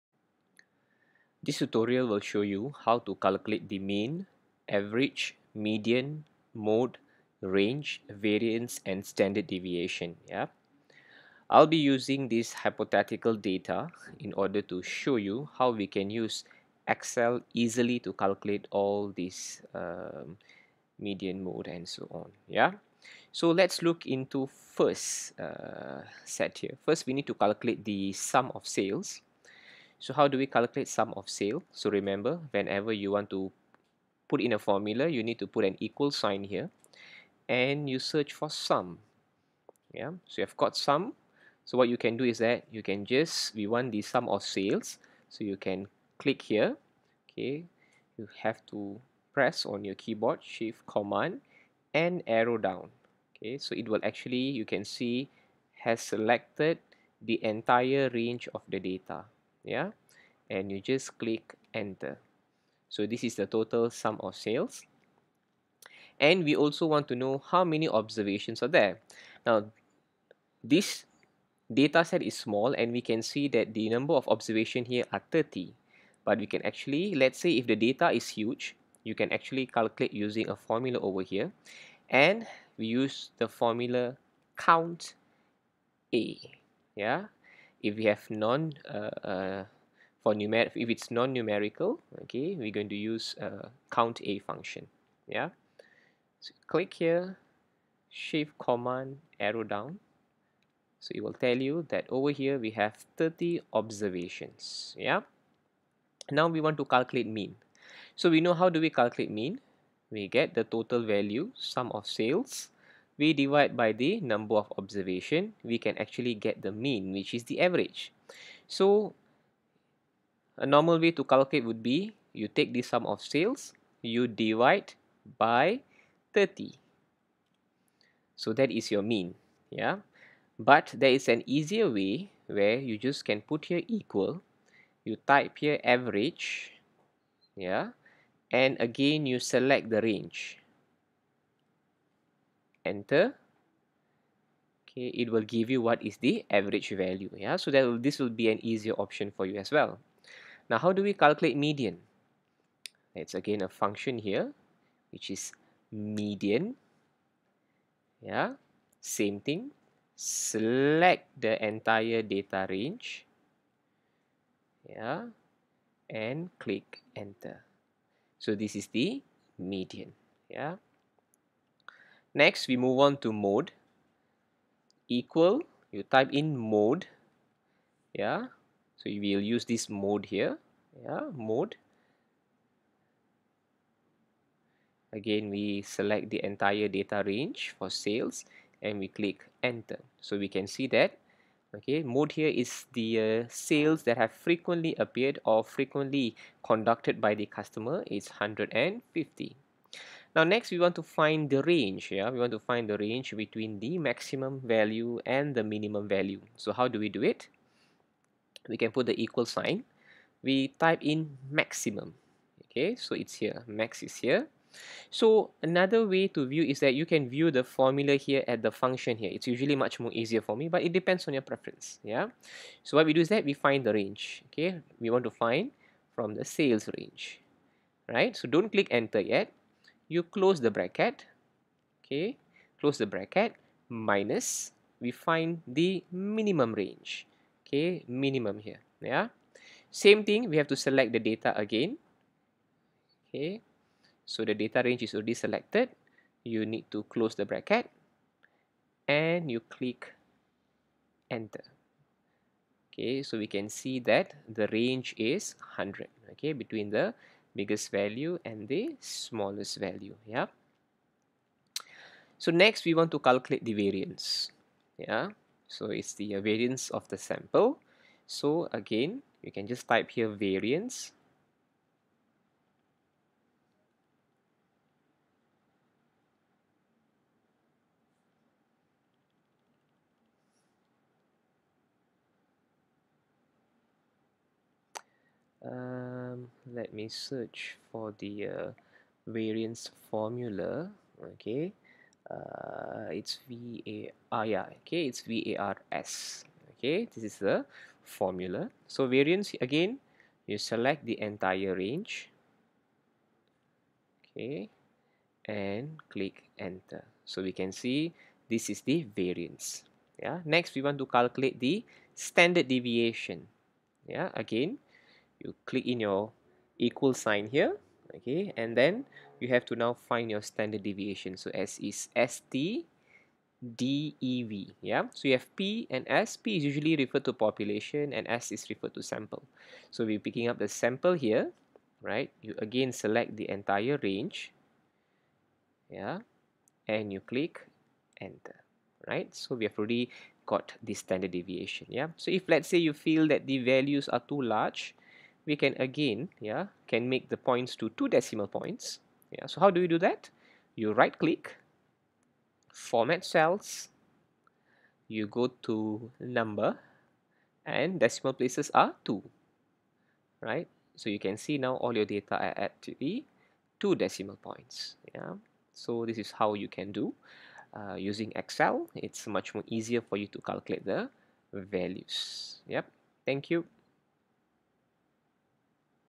Measures of Central Tendency
This video assists you on how to calculate Mean, Mode, Median, Range, Variance, and Standard Deviation using Microsoft Excel